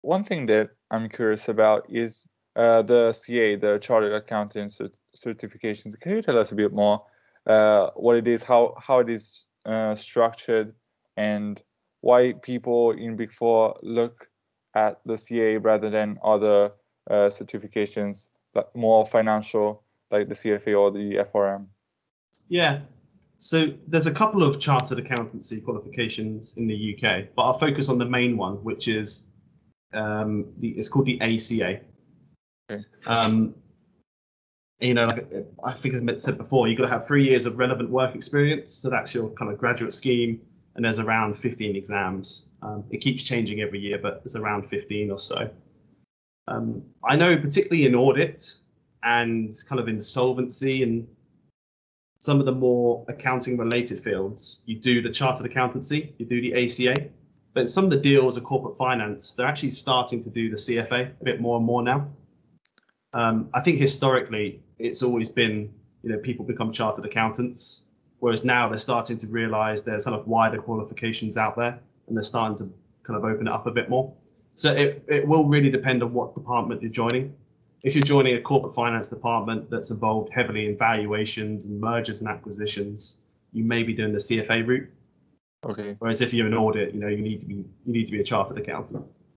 Why the Big 4 emphasize the Chartered Accountant qualification rather tan the CFA
In this video @JamesAlberts explains how the Chartered Accountant is structured, how much it takes to get it and why the Big 4 (Deloitte, PWC, EY, KPMG) want their employees to have this certification. In the end he explains what kind of roles require the CA qualification and what kind of roles rquire different certifications like the CFA